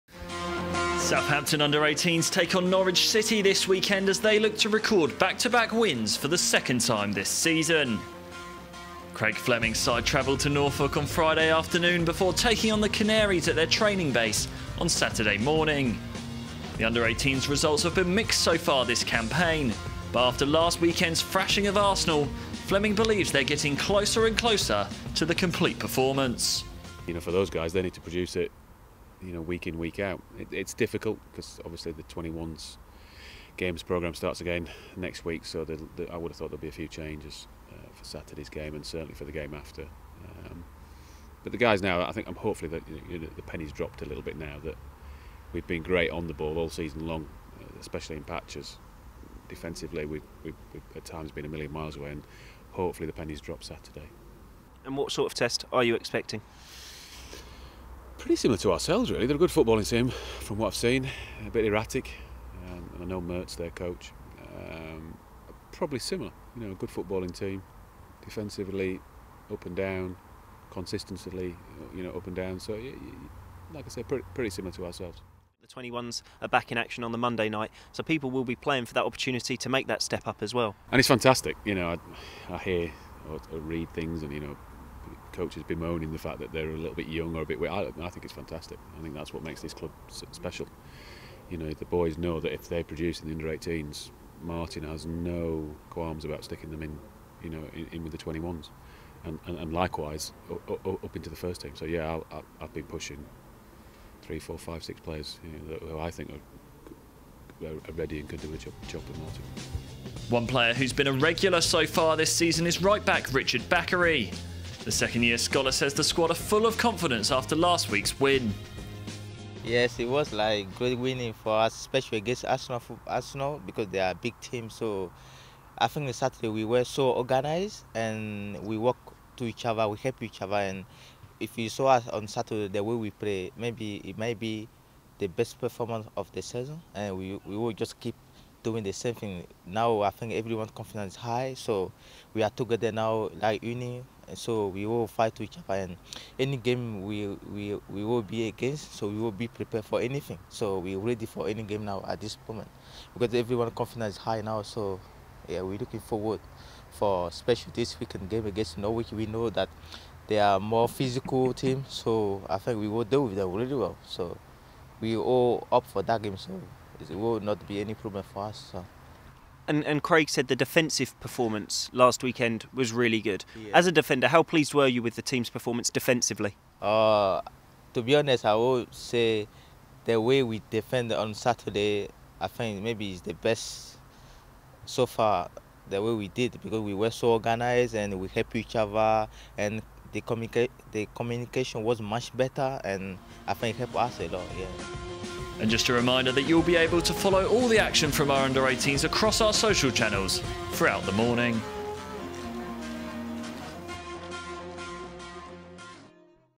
U18 PREVIEW: Fleming and Bakary out to build on Arsenal win
Southampton Under-18s coach Craig Fleming and right back Richard Bakary preview their side's trip to Norwich City in the Barclays Under-18 Premier League.